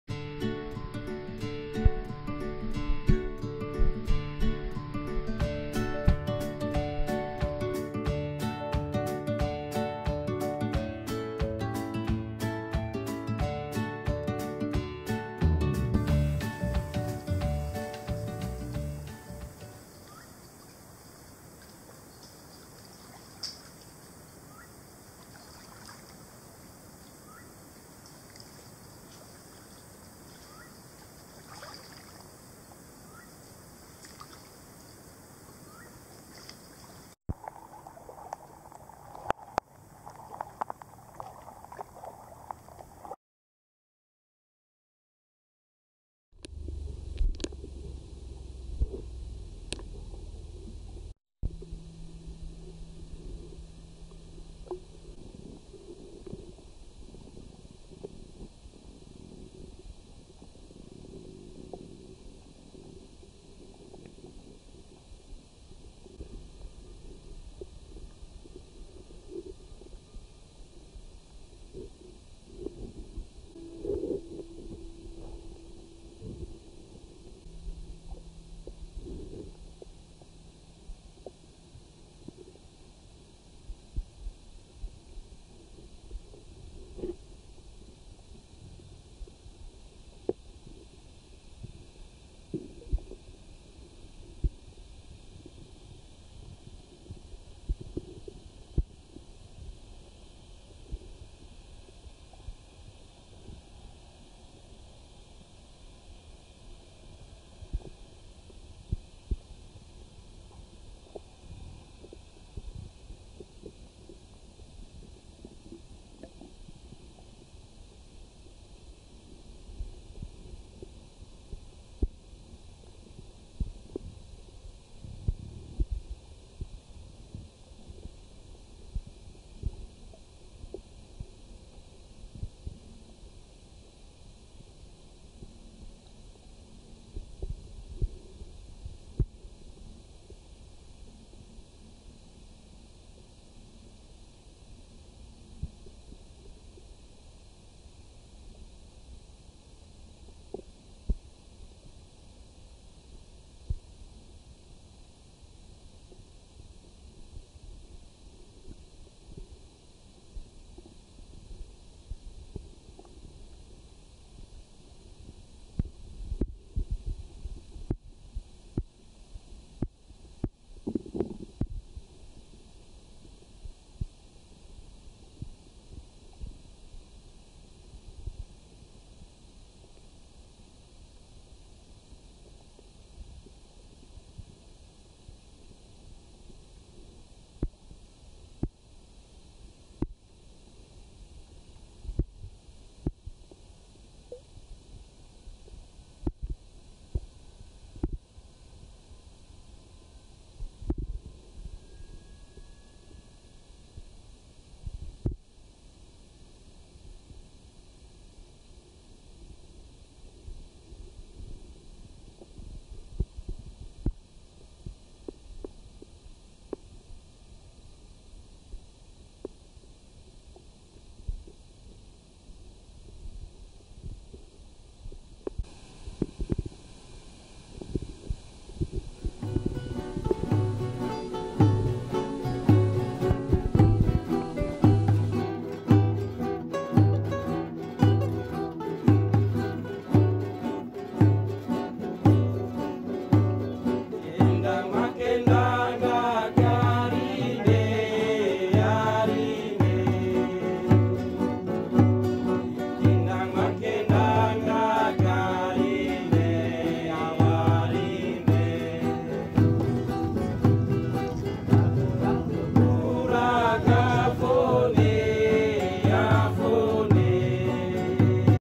Salmon-red rainbowfish shoaling amid roots of Sago palm
Salmon-red rainbowfish (Glossolepis incisus) prefer to shoal in densely roots of Sago palm (Metroxylon sagu) which are growing almost around the shore of lake Sentani. After visit the lake, i have came to my point that this plant is really vital, not only for local Papuans but also the fish, especially those native one such as rainbowfishes and its counterparts.